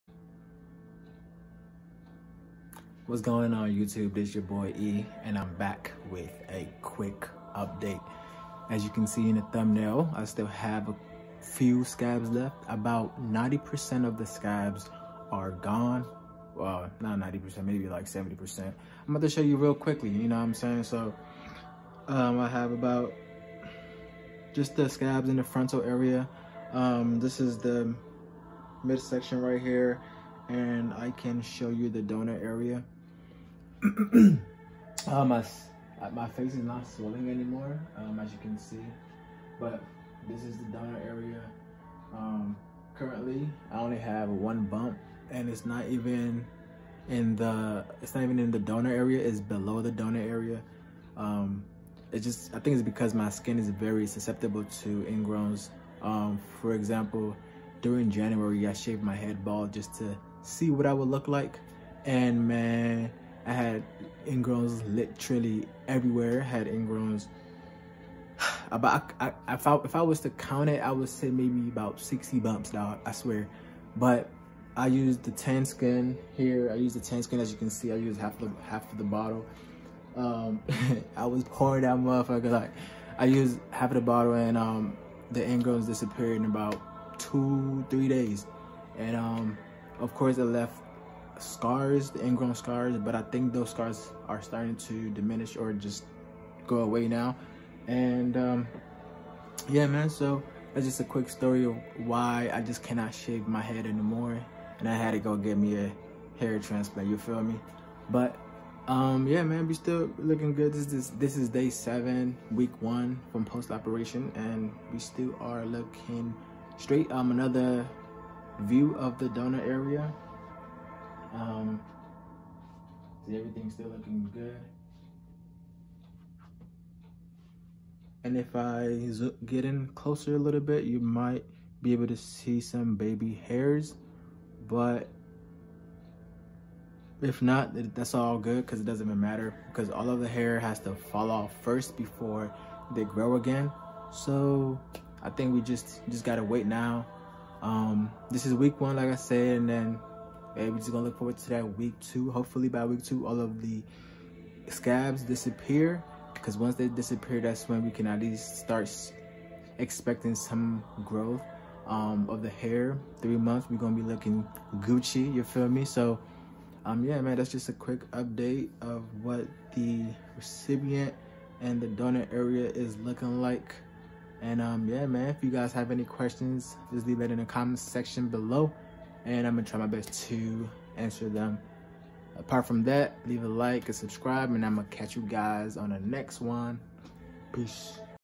Week 1 African American Hair Transplant FUE (Im still scabby)
My experience:
 Clinic: Asli Tarcan Istanbul, Turkey
 How many: 5500 grafts
 Cost:   $2500 USD
 When:   July 24th 2021
 Age:   25
 Medications: None. I will NOT be using Minoxidil nor Finasteride